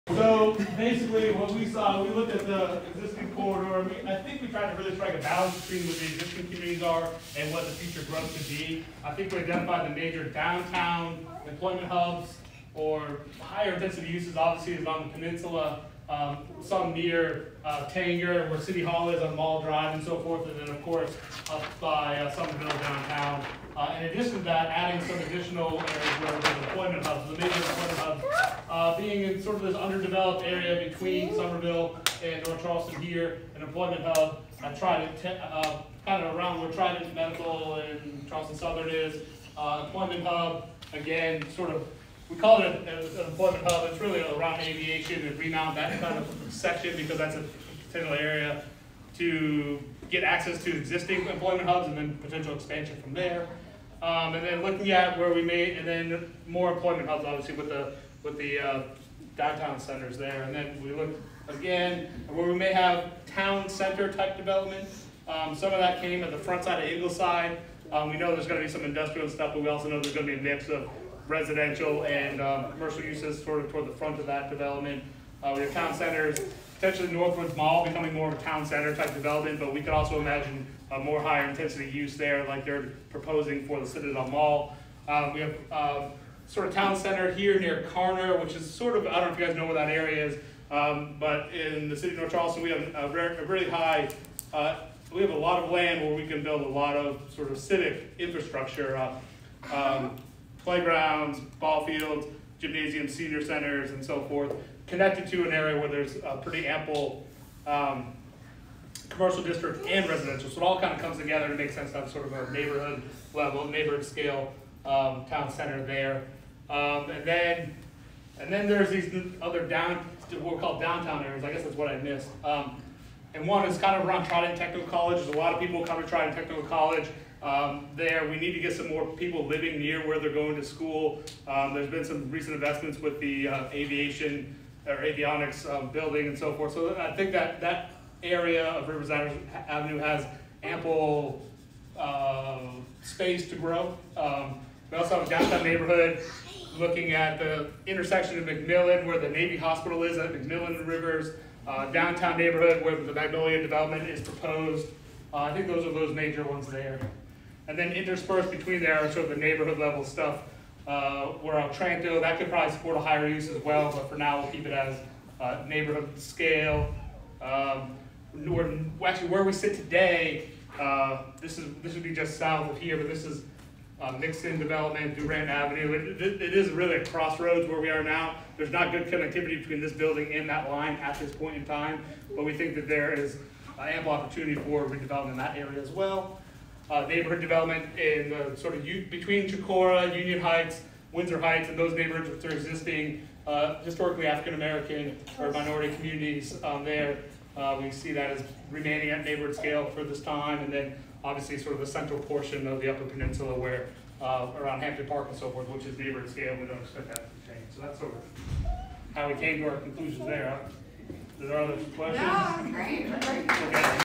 TOD Station Location Presentation
Here's a short presentation by one of our TOD community workgroups at last night's workshop giving their insights on ideal station locations along the corridor.